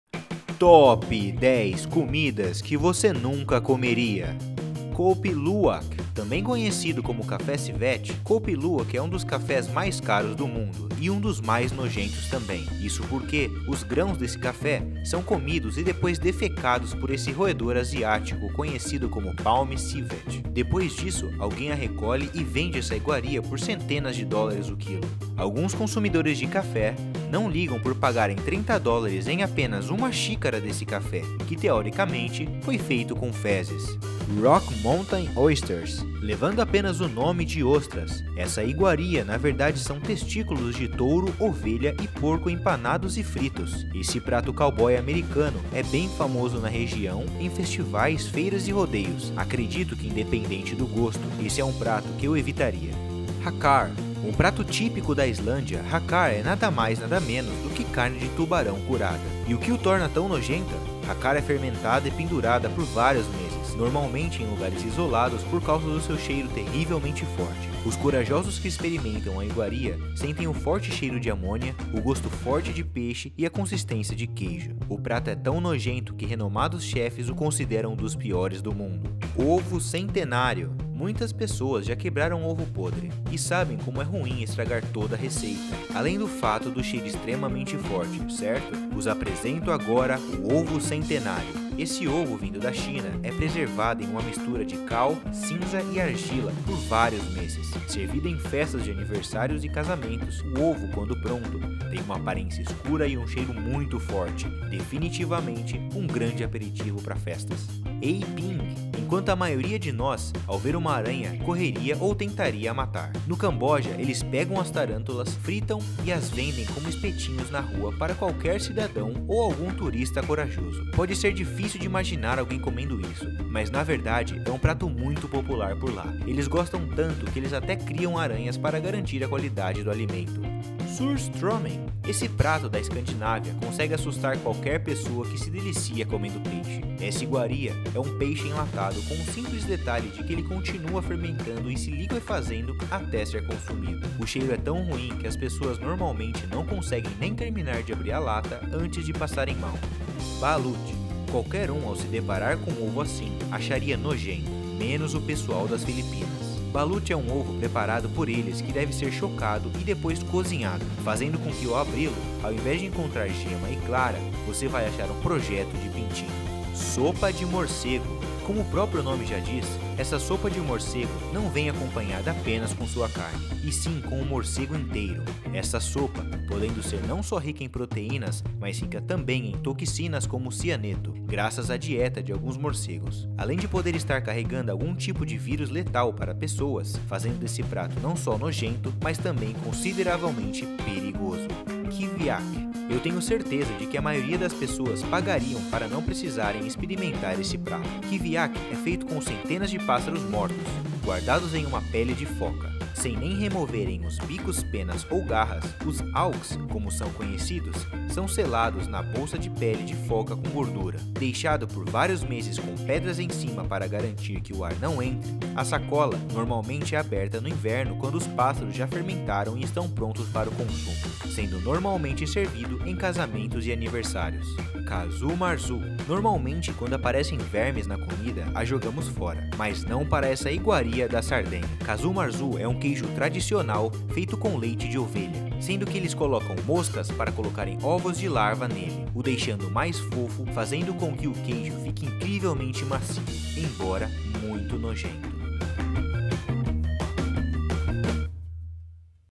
TOP 10 COMIDAS QUE VOCE NUNCA COMERIA
Tem algum tipo de comida que sua mãe ou vó faz que você fica meio com medo de comer? eu pensaria duas vezes antes de reclamar... Aposto que não são piores do que os itens dessa lista.

E no top 10 de hoje estão as piores, mais nojentas, mais fedidas, horríveis, bizarras e de tirar o apetite que você já viu:
10. Kopi luwak
9. Rock mountain oysters
8. Hakarl
7. Ovo centenário
6. A-ping
5. Surstromming
4. Balut
3. Sopa de morcego
2. Kiviaq
1. Casu Marzu